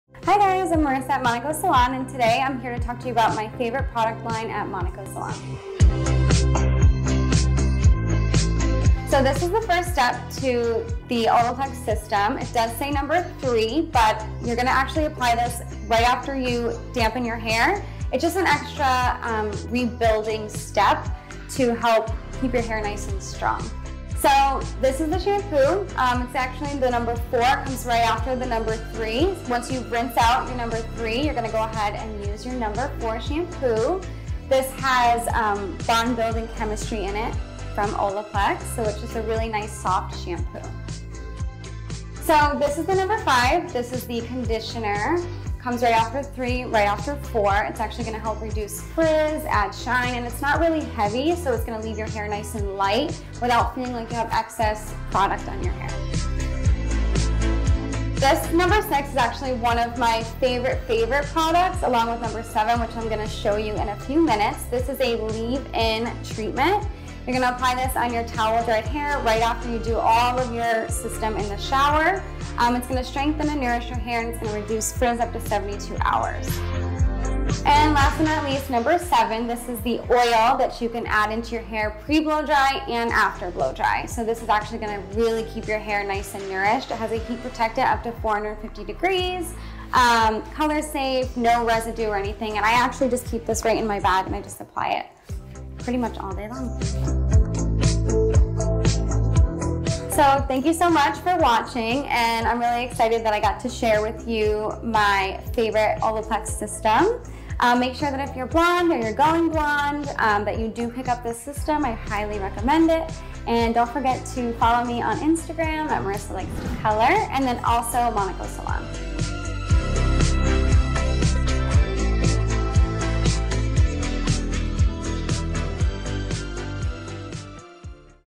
My favorite products | Olaplex | Monaco Salon
Thinking of going blonde? @marissalikestocolor walks you through her favorite product line @olaplex for keeping your blonde shiny and healthy!